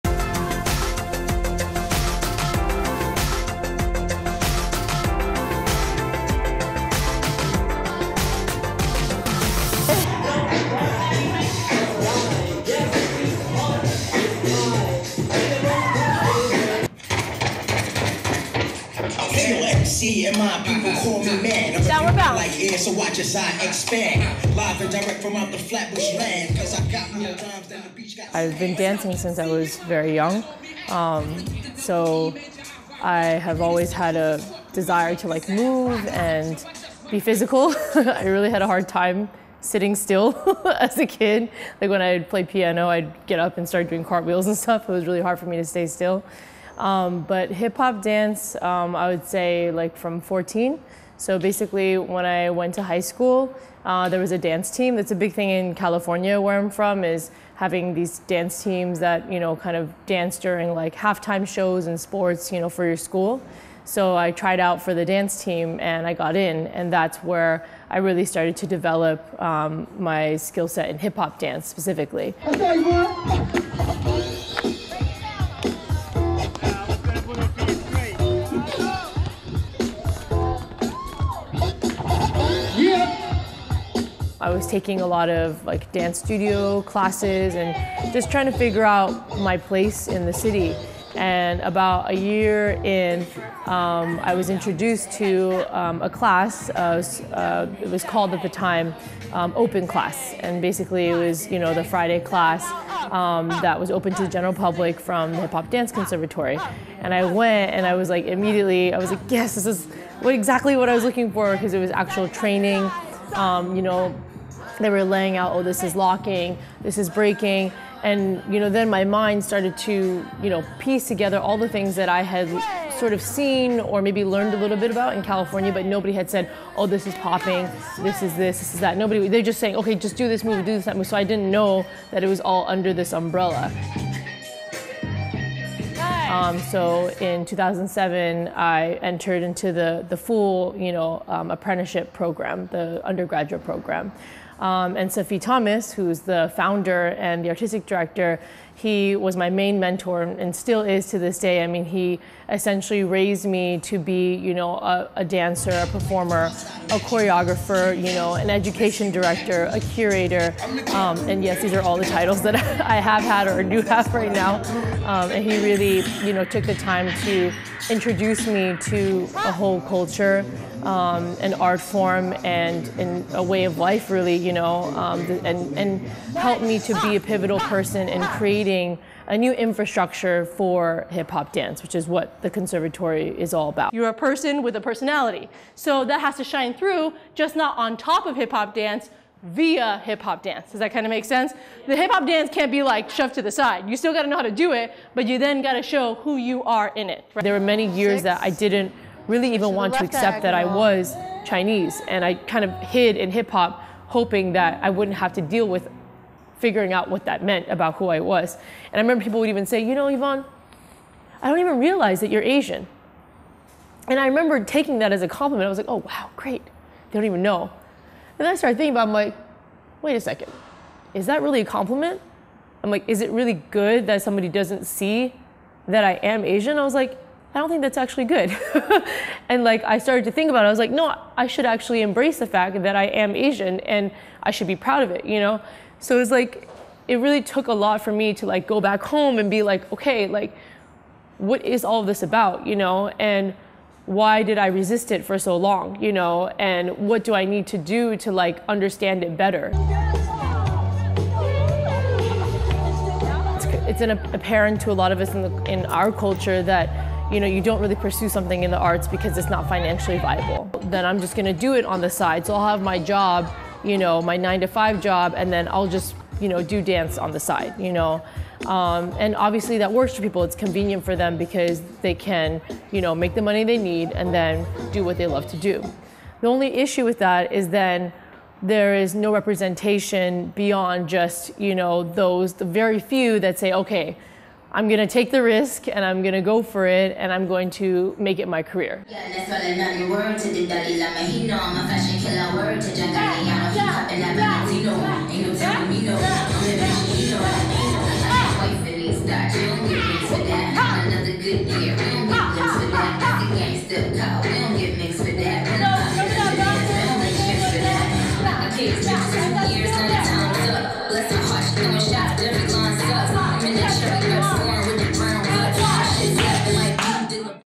Occupation Nation- Yvonne Chow
Yvonne H. Chow is a Chinese American Hip-Hop Dancer, Choreographer, and Teacher of the non-governmental organization (NGO), H+ | The Hip-Hop Dance Conservatory. Raised in California, she has had a passion for Hip-Hop culture and for supporting more people who are interested in being a part of Hip-Hop culture, especially Asian (Americans). Chow teaches, performs, and lectures with the Founder and Artistic Director, Safi A. Thomas at various universities, corporations, the United Nations, and more about how to respectfully and knowledgeably engage with and contribute to the legacy of Hip-Hop.